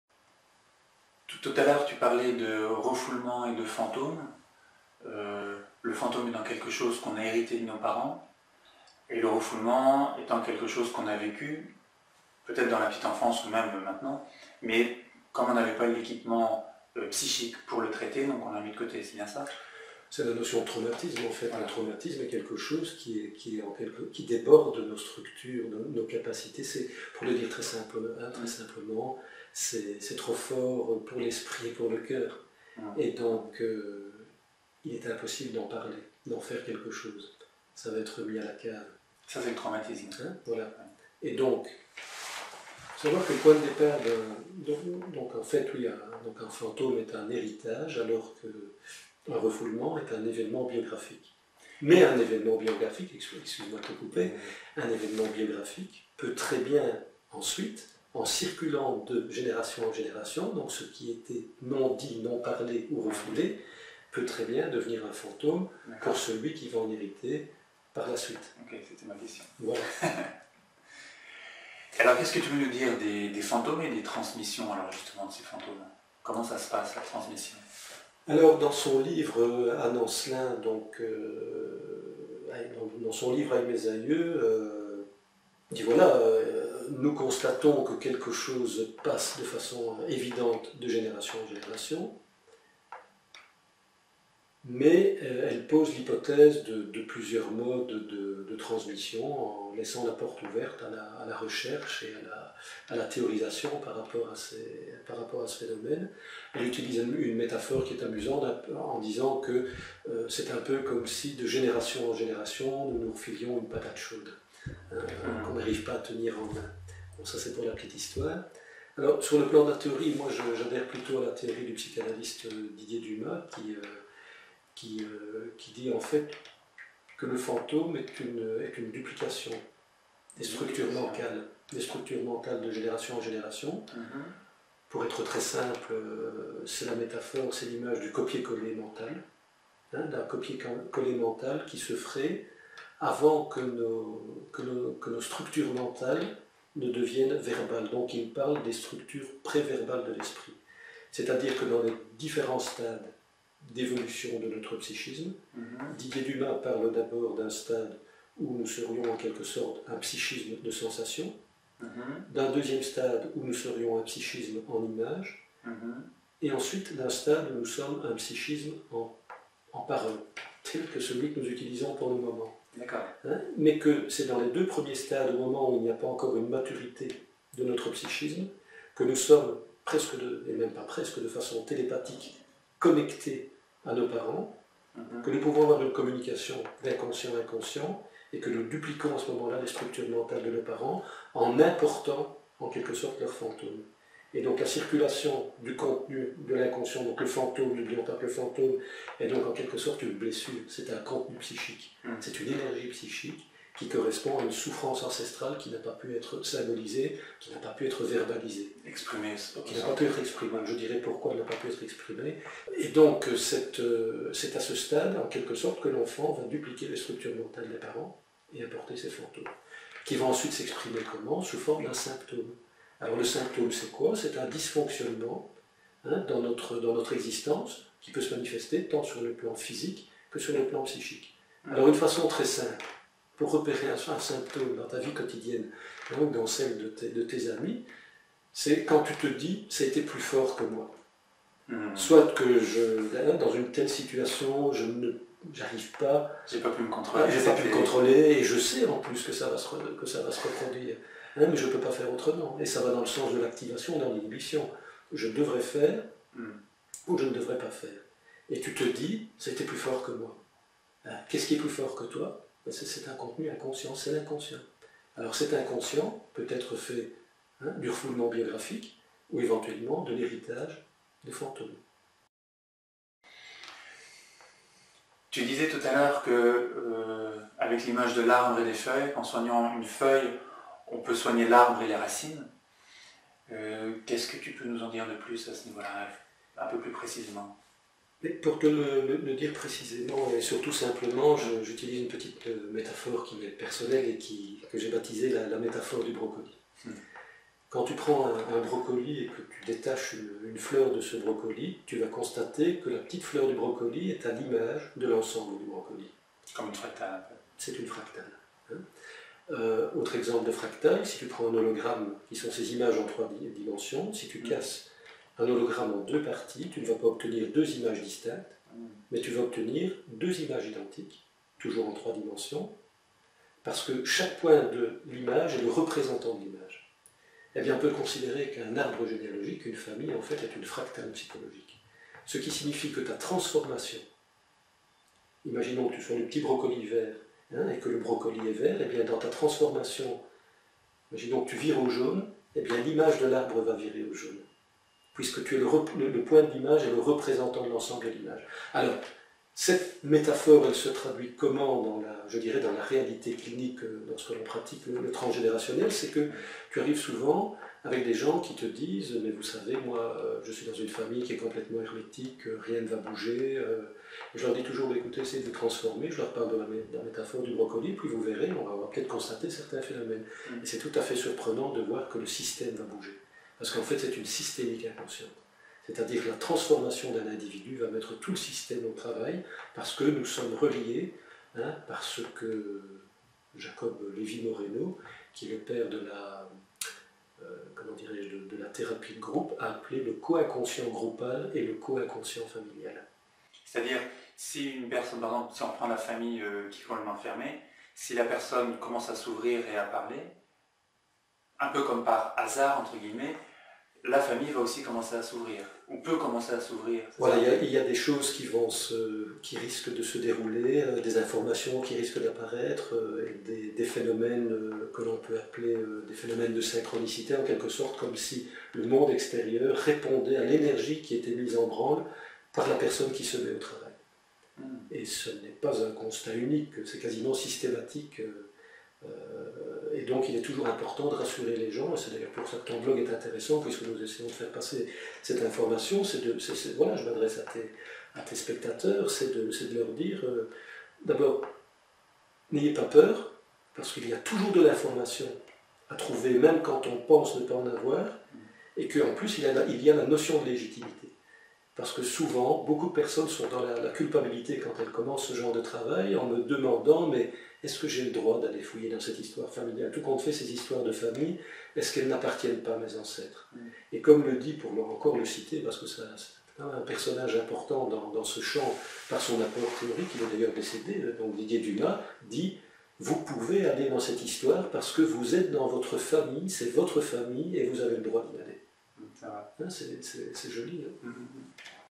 La psychogénéalogie Interview de Pierre Ramaut par Philippe Chevaux 2.1
Pour bien vivre, nettoyez vos morts.
Le point sur la psycho-généalogie, ce que c'est et ce qu'elle apporte. Interview de Pierre Ramaut psychanalyste à Mons, par Philippe Chevaux du blog Apprendre sur soi et avancer.